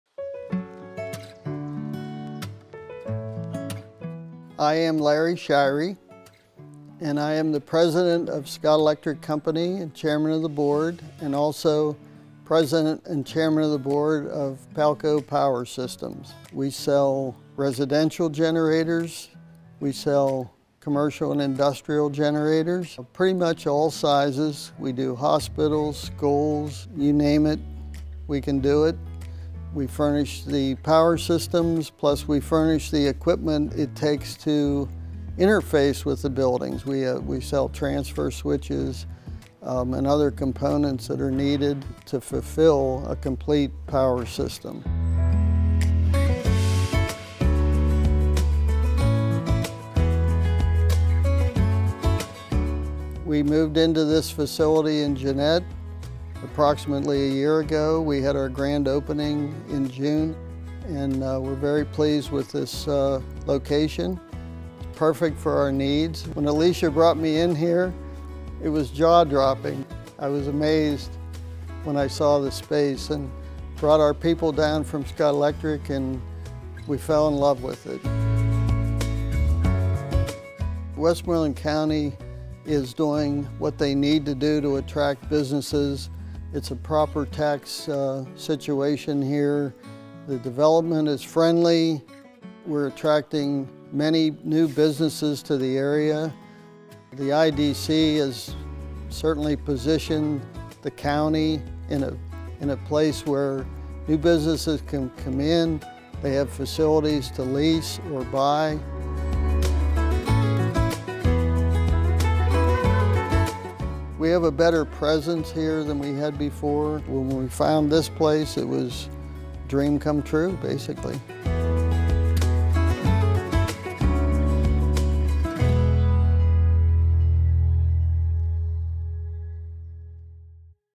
Spotlight Westmoreland: Palco Power Systems
Larry Shirey, president of Palco Power Systems, recounts how Palco Power Systems found their ideal location in Jeannette Industrial Park and explains why business-friendly Westmoreland County is a great option for any company.